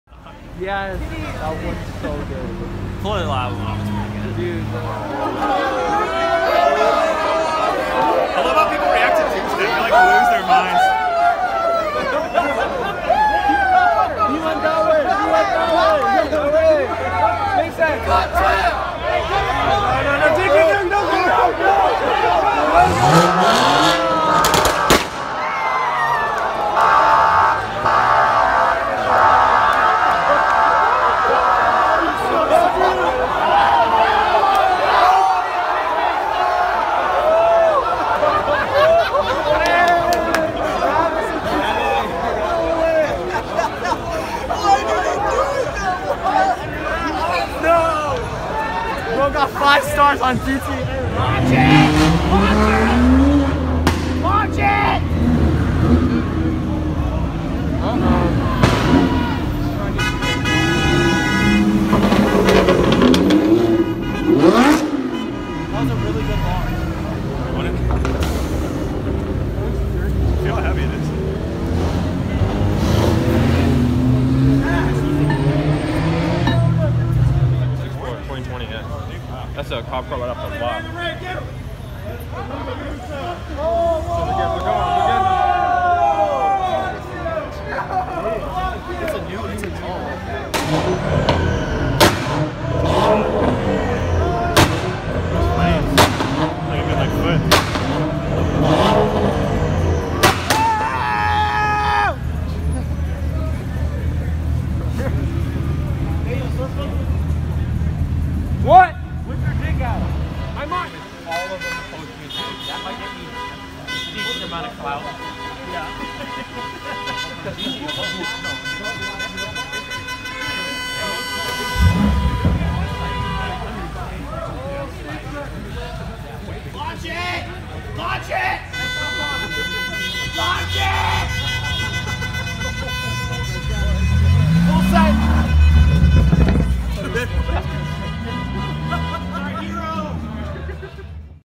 Car Rally CHAOS Ensues in Beverly Hills, Cops Everywhere
It’s been a while since we’ve uploaded. But here is a little compilation of Friday night’s Malibu Autobahn to Beverly Hills. We will be trying to post more compilations in the future. Enjoy!